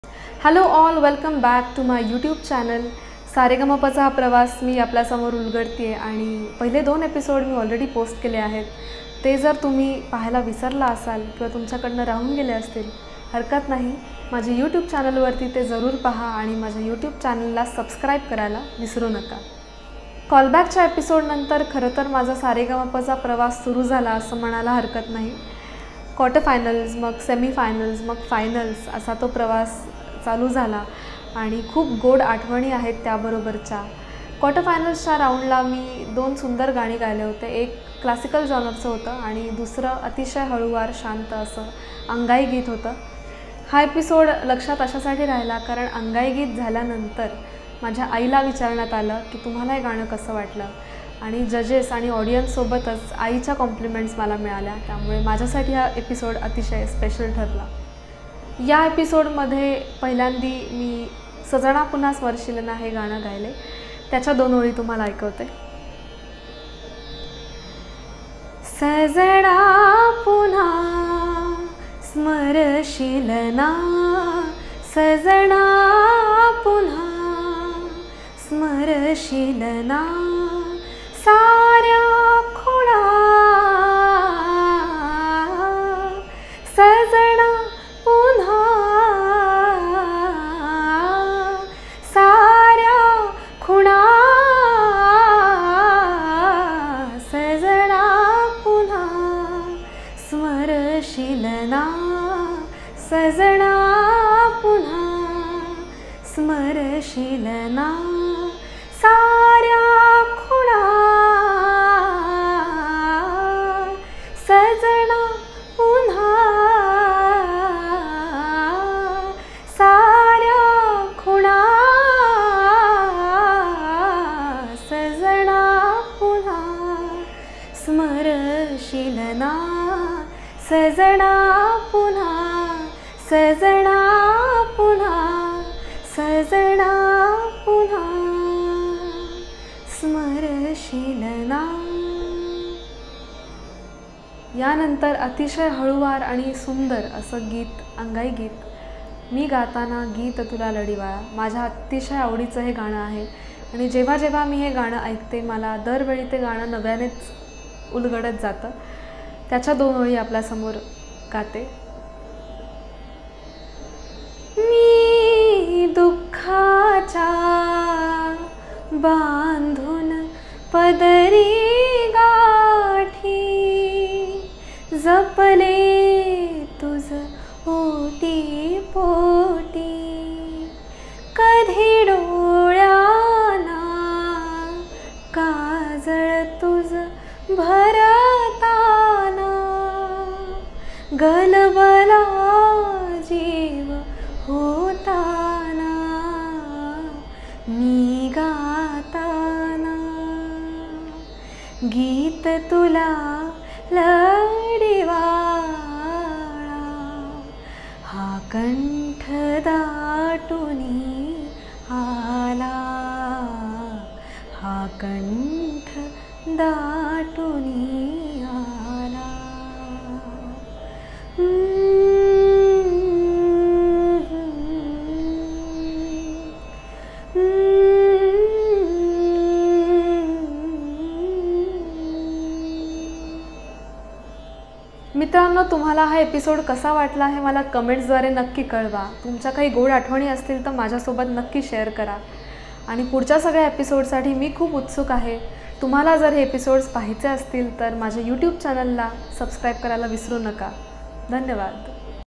Episode 3 | Journey to L’ll Champs | Shamika Bhide
Welcome to the YouTube Series
‘Journey to L’ll Champs’.

Episode 3 : Quarter Final Round & the experience of Zee Marathi SAREGAMAPA Little Champs 2008.